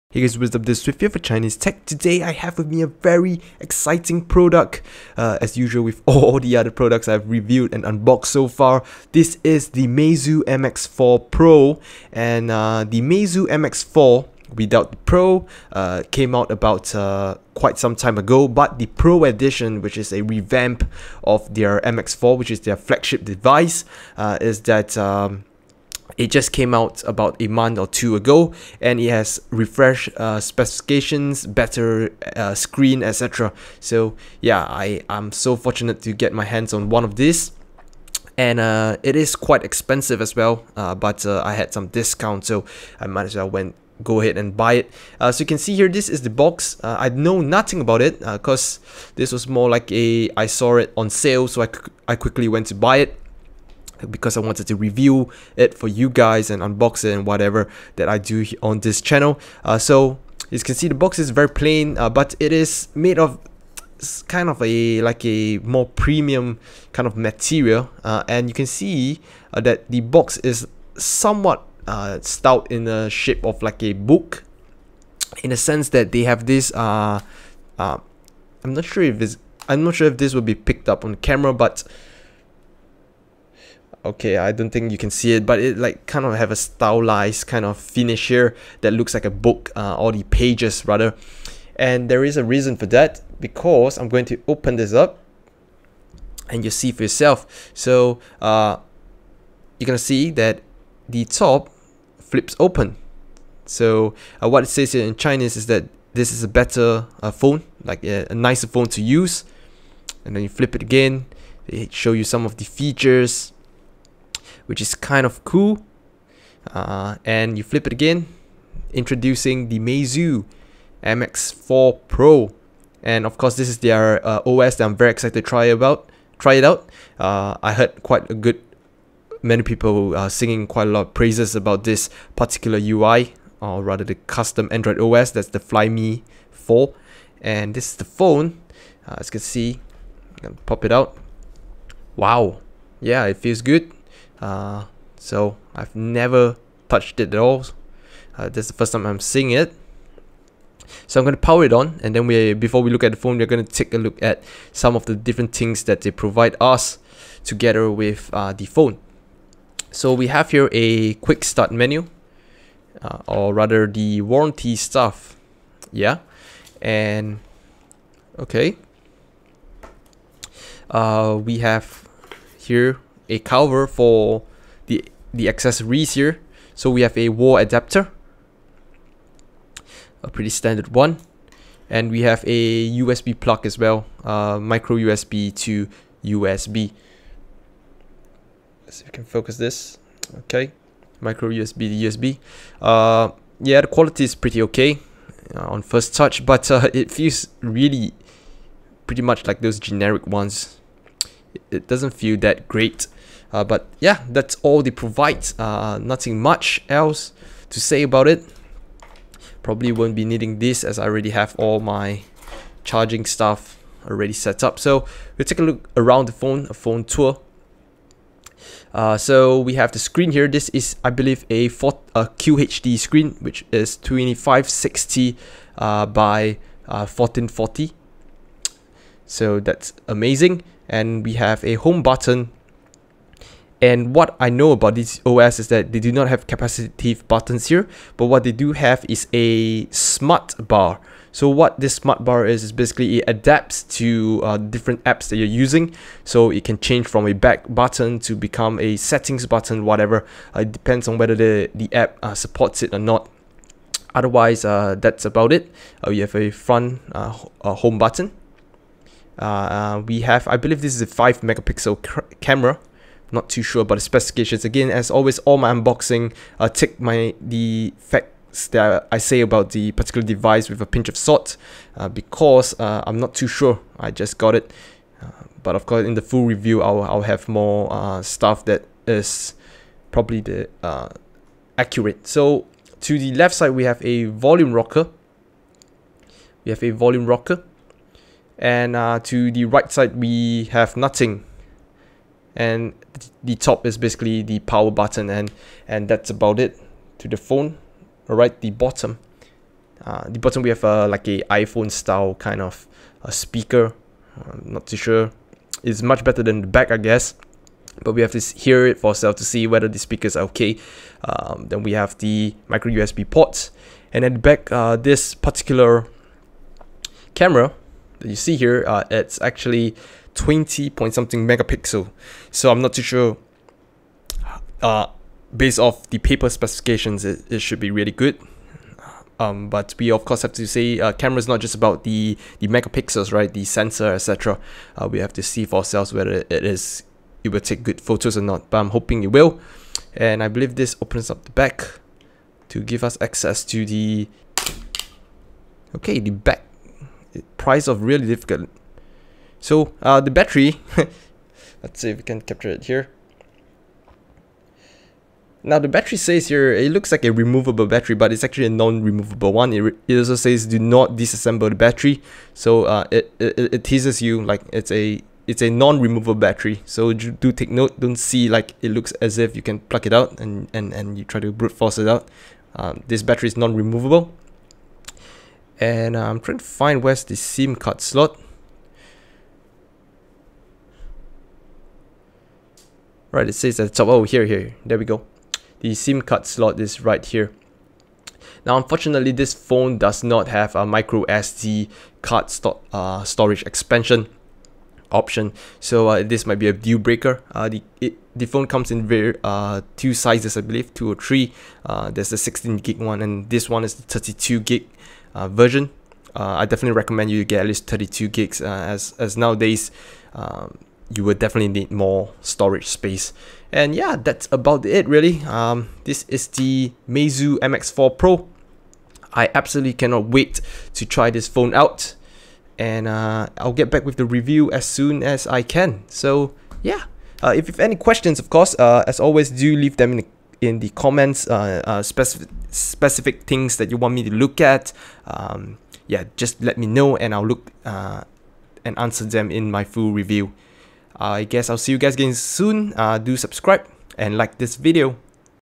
Meizu MX4 Pro Unboxing & Overview
One more step towards reviewing all major Chinese brands! This time around I have with me the Meizu MX4 Pro. Another flagship device with some top of the line specifications. As always, the things I say in the video is totally uninformed so do take it with a pinch of salt.

Full review up in a week's (or two) time! At the moment if you have any specific things you want me to look into, do leave them in the comments!